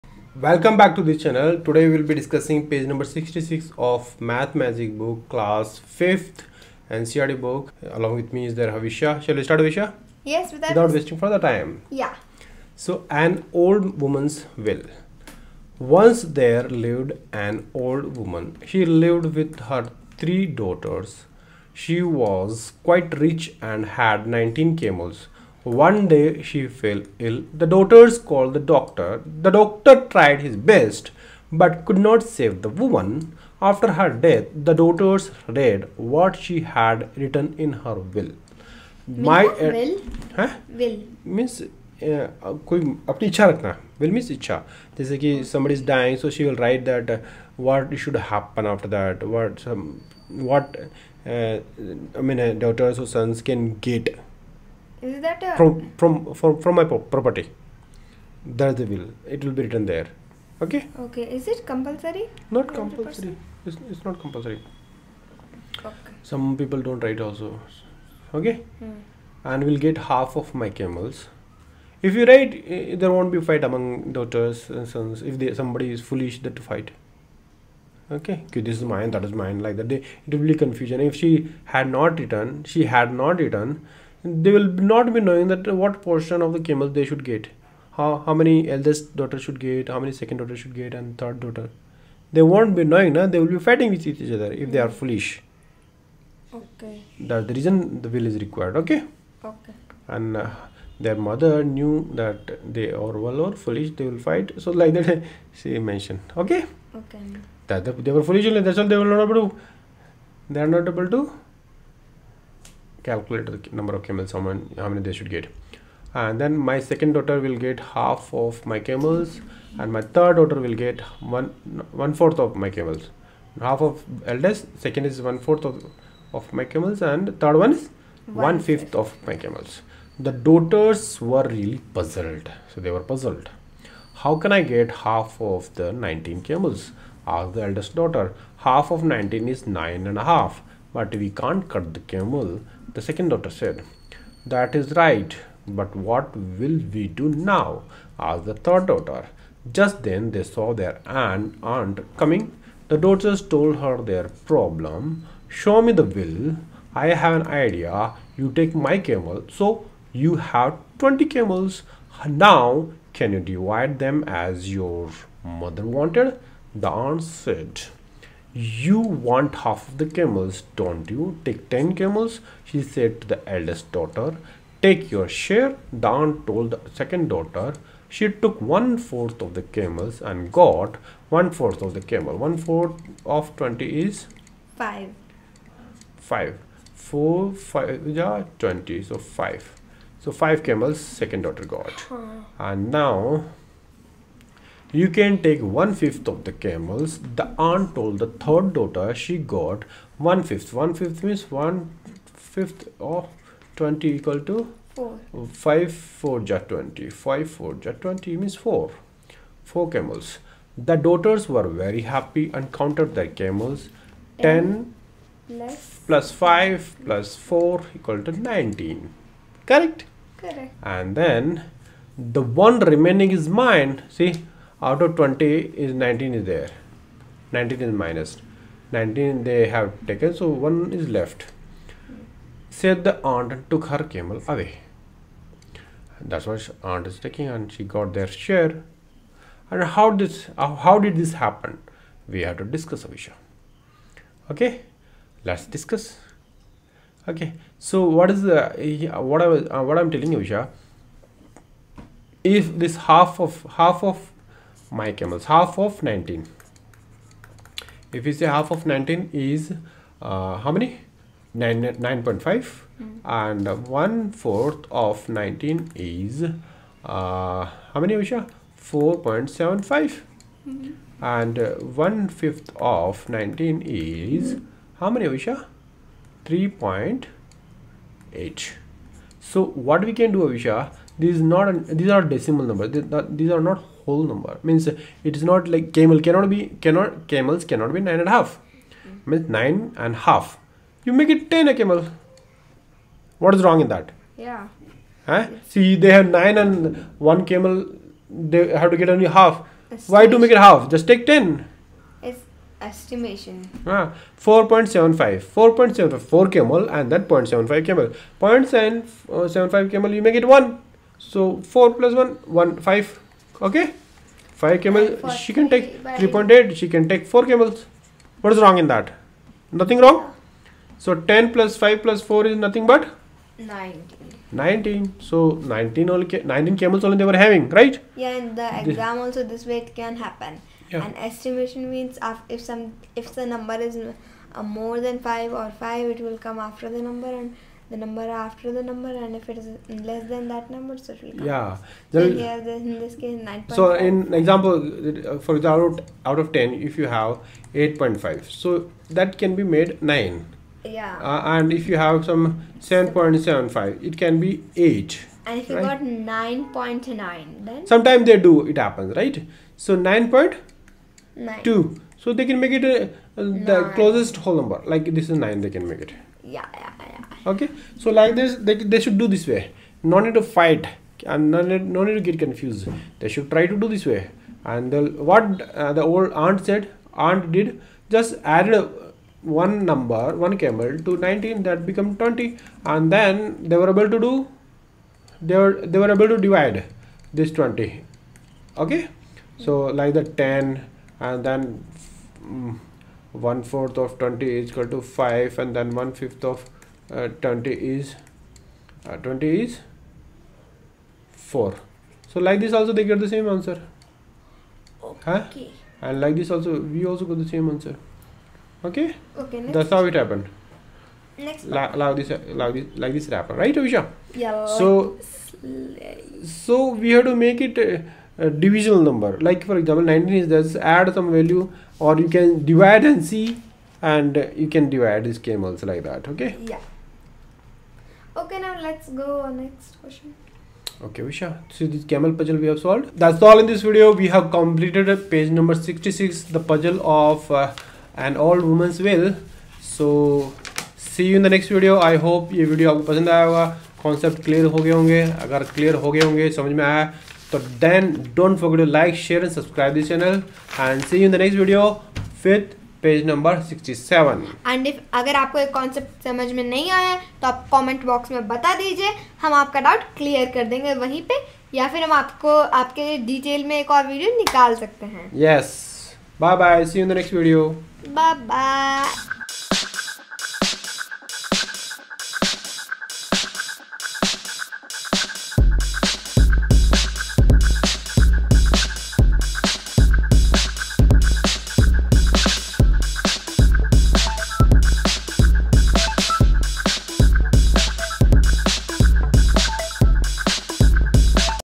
NCERT Class 5 Maths Chapter 4 II Parts and Wholes II Page 66 II Math Magic
This video explains in detail chapter 4 Parts and Wholes ? Page no. 66 of Maths (Math Magic) of Class 5. I encourage you to watch this video completely.